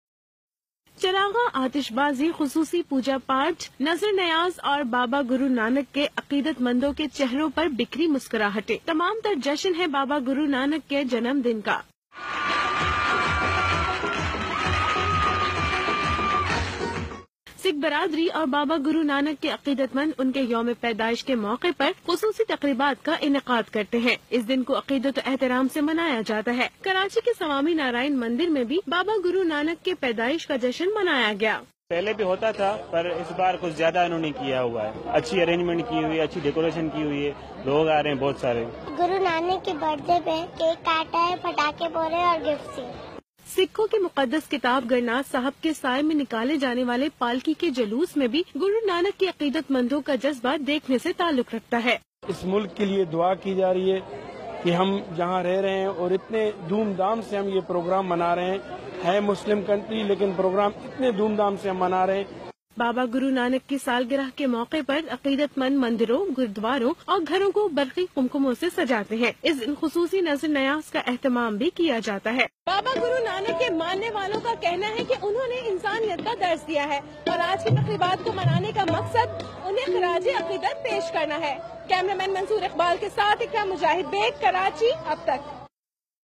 Shri Guru Nanak Dev G Birthday celebrities in Pakistan
ਪਾਕਿਸਤਾਨ ਦੇ ਕਰਾਚੀ ਵਿੱਚ ਮੁਸਲਮਾਨਾਂ ਨੇ ਮਨਾਇਆ ਸ੍ਰੀ ਗੁਰੂ ਨਾਨਕ ਦੇਵ ਜੀ ਦਾ ਪ੍ਰਕਾਸ ਉਤਸਵ